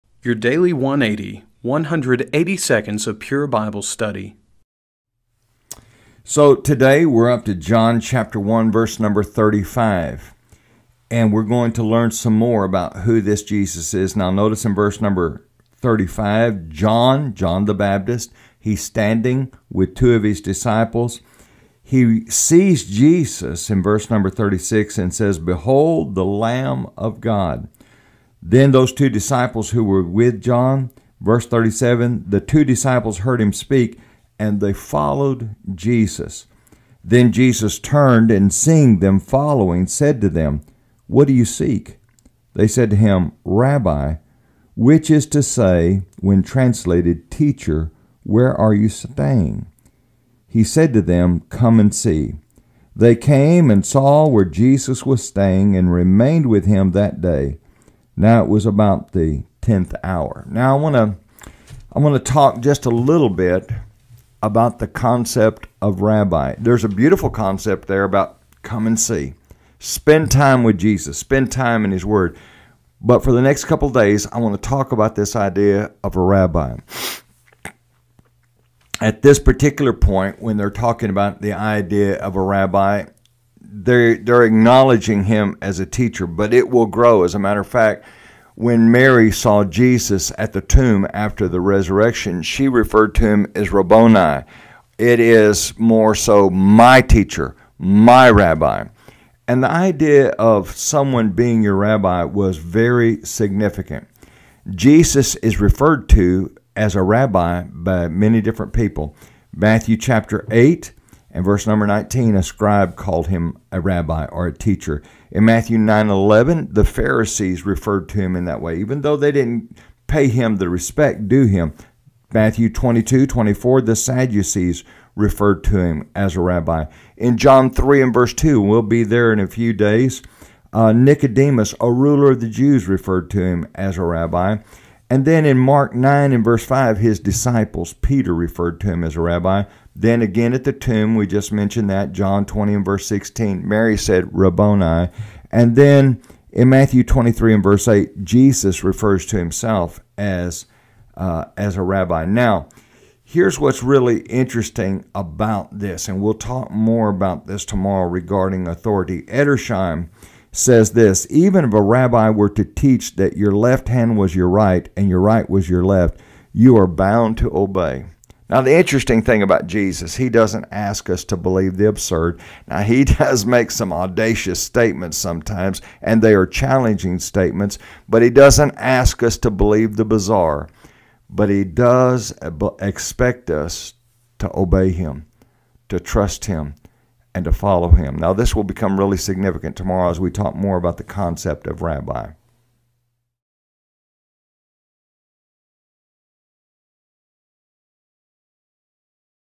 The Gospel of John: Jesus the Rabbi
Your Daily 180: Tuesday, February 6th, 2024

The historical background of a rabbi in the 1st Century reveal fascinating insights to the Jewish view on their teachers. At times, however, they could be very misleading. Why was Jesus referred to as a rabbi then by the early disciples?